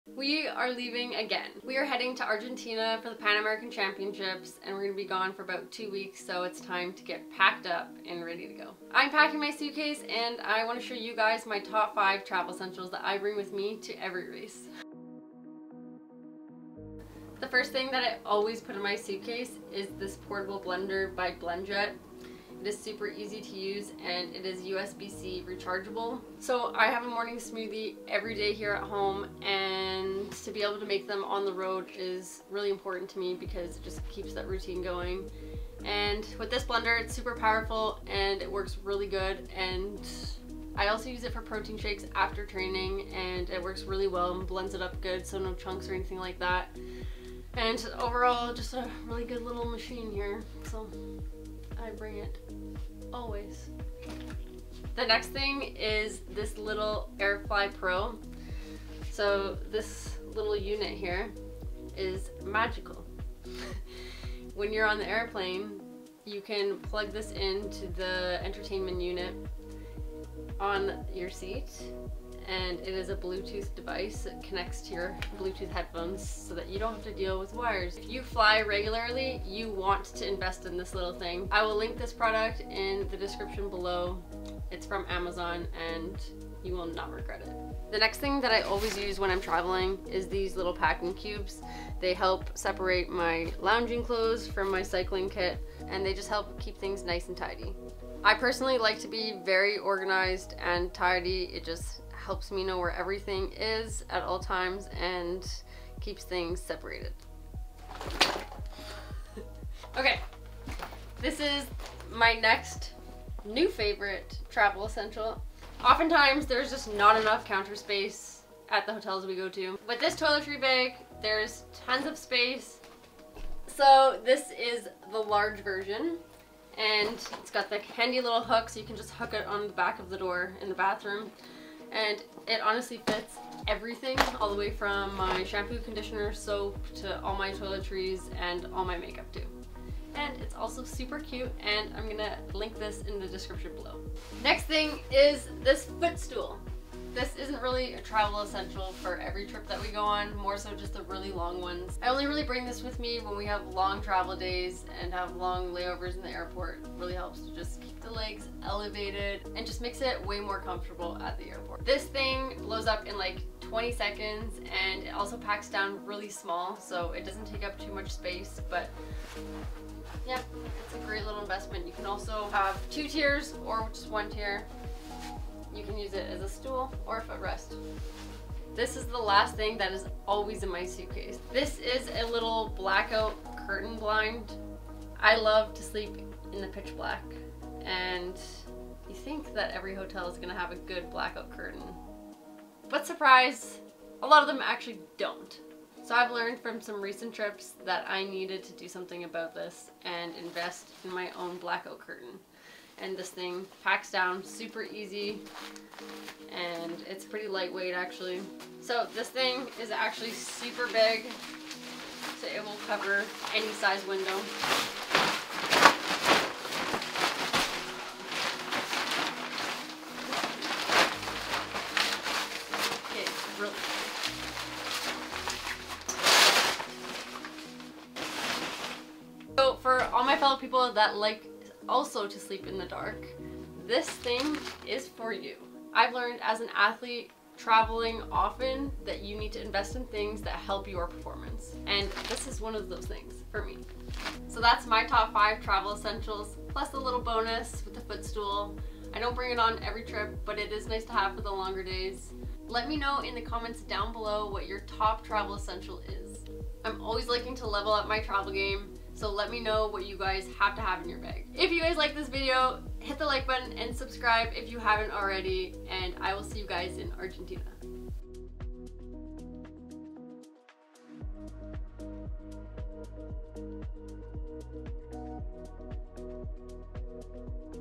MY TOP 5 TRAVEL ESSENTIALS AS A PRO ATHLETE | Packing for Pan American Championships
Time to head across the world for another competition! Come pack my suitcase with me and see my top 5 travel essentials when I'm headed to a competition!

THE AIRFLY PRO!! A must have for flying!
The Blow up Foot Stool!